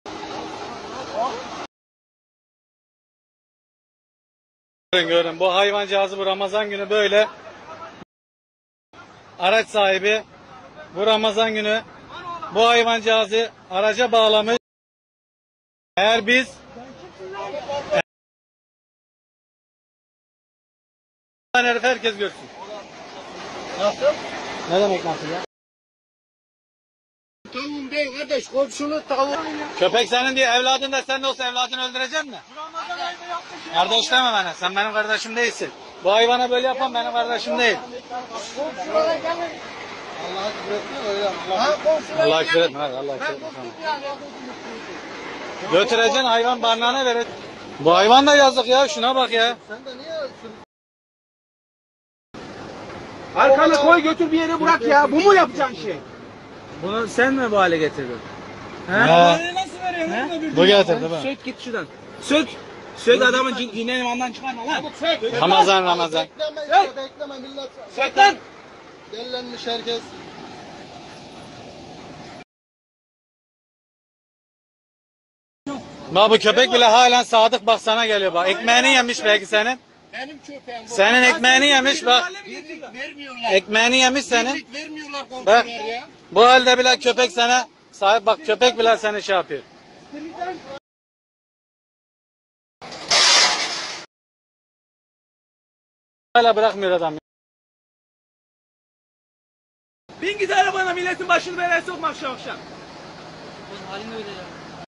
Çekicinin arkasına bağlayıp sürükledi... İsyan ettiren olay!
Adana'da, komşularının tavuklarına zarar verdiği gerekçesiyle köpeğini çalıştığı şirketin çekicisinin arkasına zincirle bağlayarak sürükleyen şoföre, görenler tepki gösterdi.

Adana'da özel bir şirkete ait çekicinin arkasına zincirle bağlanarak sürüklenen köpeğin, içler acısı hali, görenleri isyan ettirdi. Otomobille çekicinin önünü kesenler, şoförle tartışmaya başladı. Cep telefonuyla görüntülenerek sosyal medyada paylaşılan olayda, köpeğin kanlar içerisinde olduğu görüldü. İsmi açıklanmayan şoförün, köpeğin kendisine ait olduğunu, komşularının tavuklarına zarar verdiği gerekçesiyle böyle bir ceza vermek istediğini savunduğu öğrenildi. Sosyal medyada hızla yayılan olaya vatandaşlar büyük tepki gösterdi.